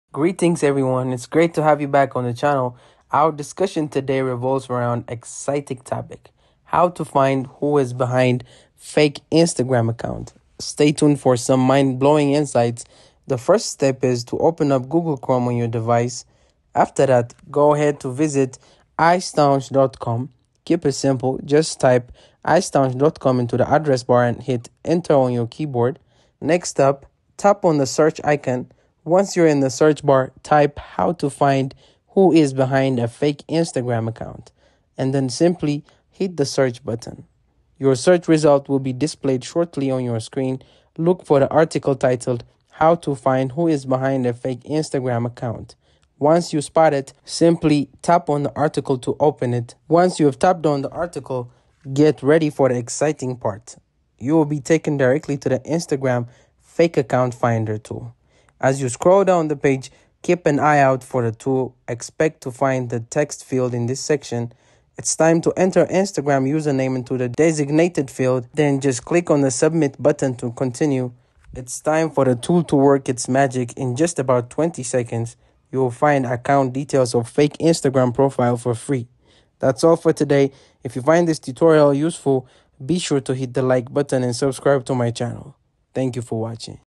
How to Find Who is Behind a Fake Instagram Account (New Method 2025)
Find the Real Person behind a Fake Account (new Easy method)

In this video I'll show you How to find out who is behind fake Instagram account (AT LEAST SOME INFORMATION). The method is very simple and clearly described in the video. Follow all of the steps & Find the Real Person behind a Fake Account || 2025